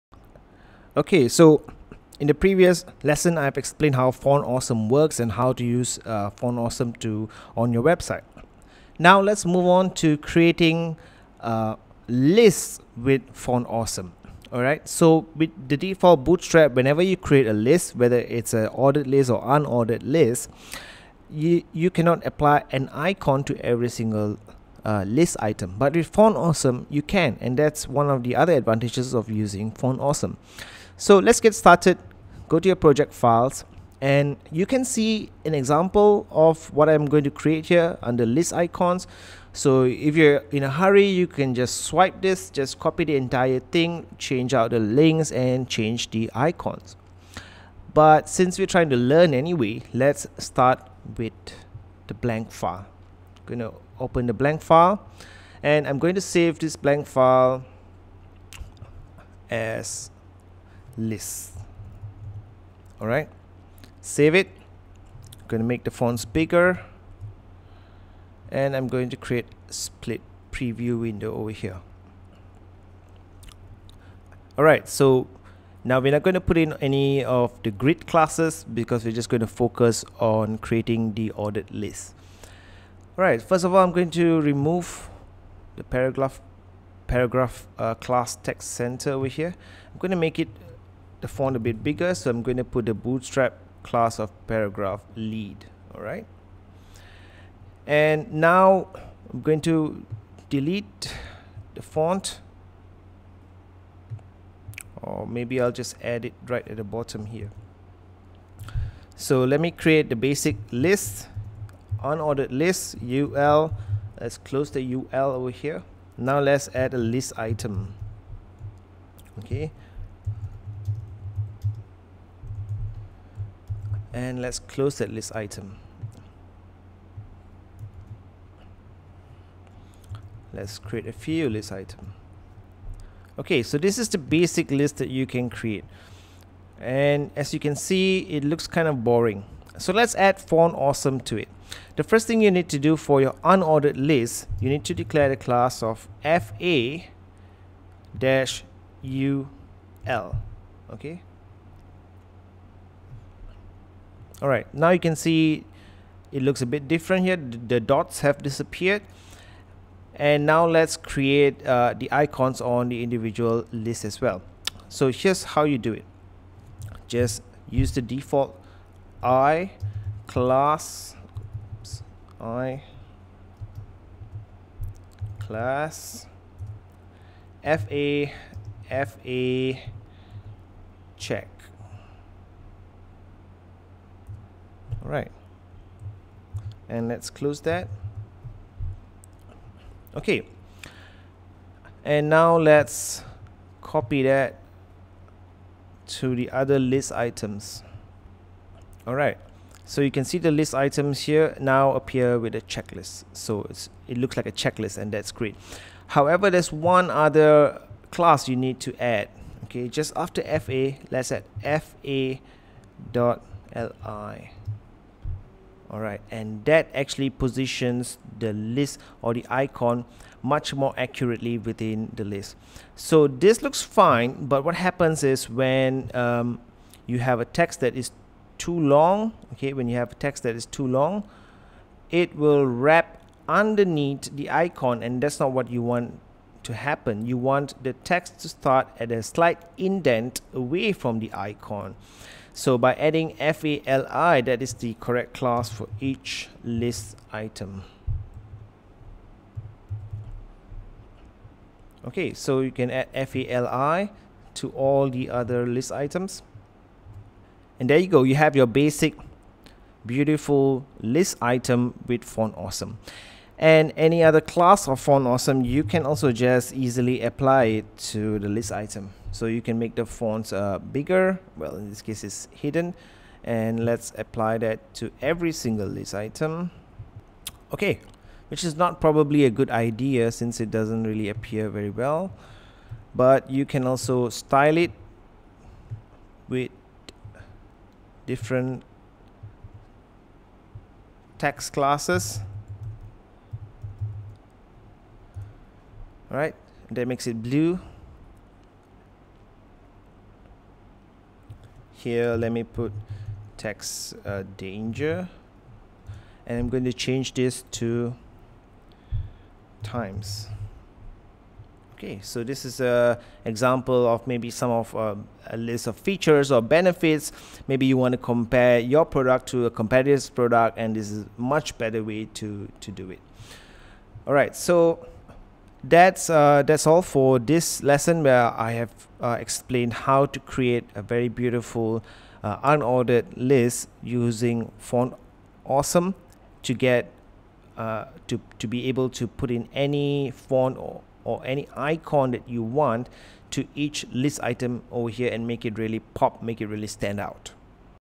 Bootstrap Tutorials Part 6 | Font-Awesome List
this is a series of video which cover the bootstrap tutorials.
 this video cover fonts awesome list.